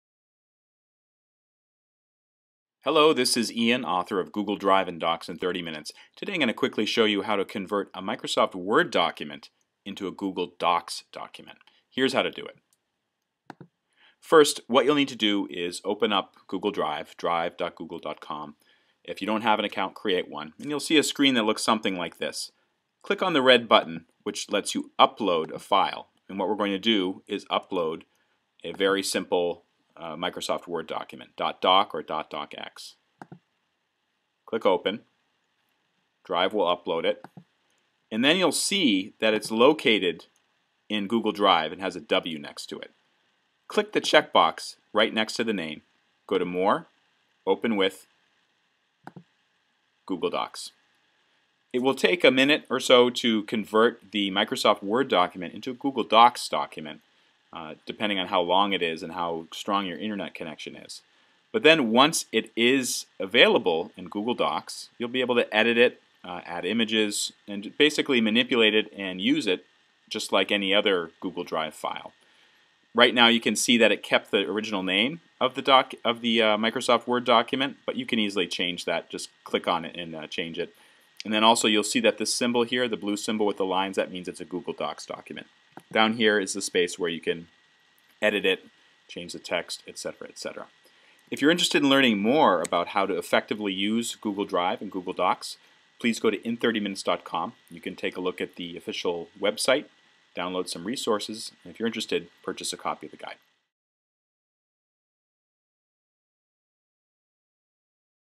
Convert .docx to Google Docs (2 minute video)
In less than two minutes, learn how to quickly convert a Microsoft Word .docx file to Google Docs. It's a great resource for people moving from Microsoft Office to the Google Drive suite. The narrator is Ian Lamont, author of Google Drive & Docs In 30 Minutes Also, "Likes" are greatly appreciated. Thank you!